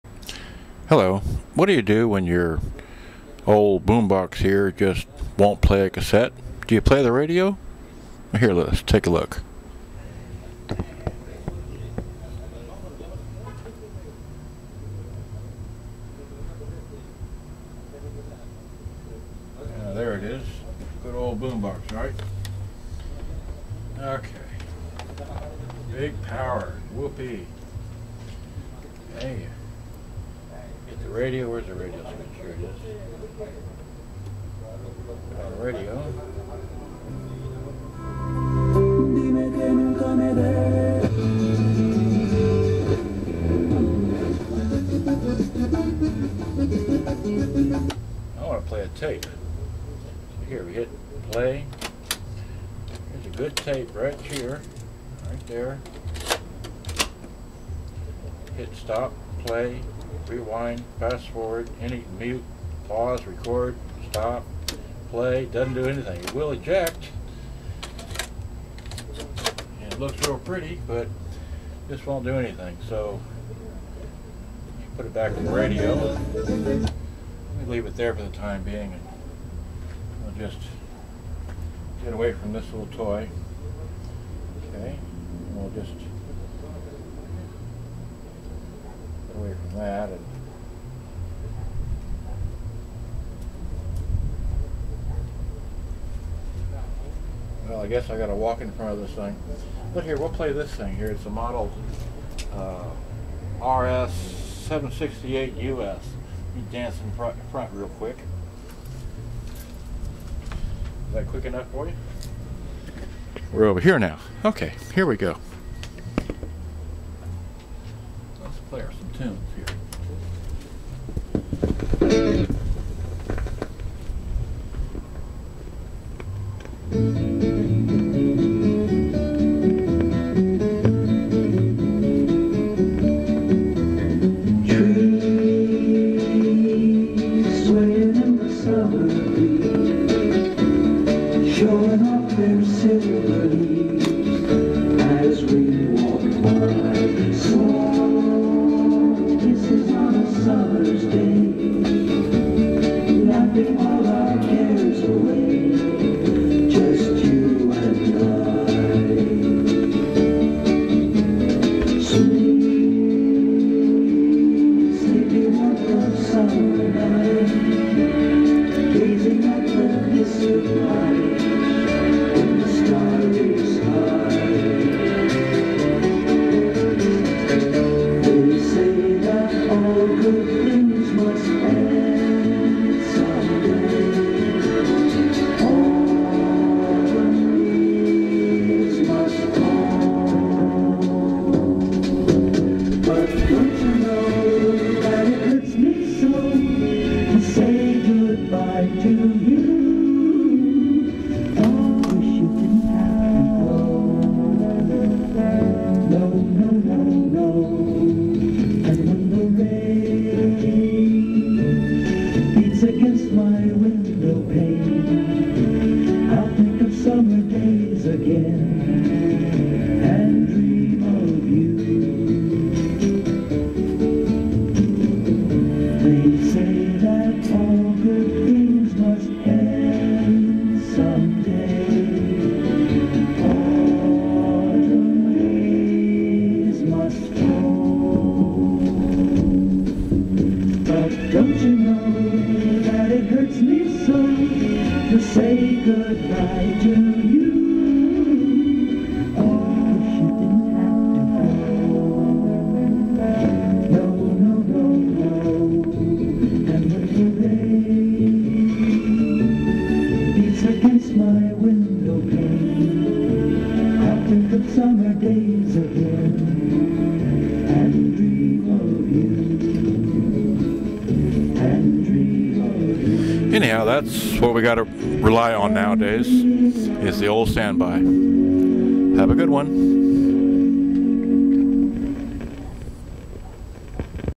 Panasonic RS-768US Reel to Reel Fallback to when Your JVC RC-M90C Boombox Fails Playing Tape M90
Panasonic RS-768US Fallback to when Your JVC RC-M90C Fails Playing Tape M90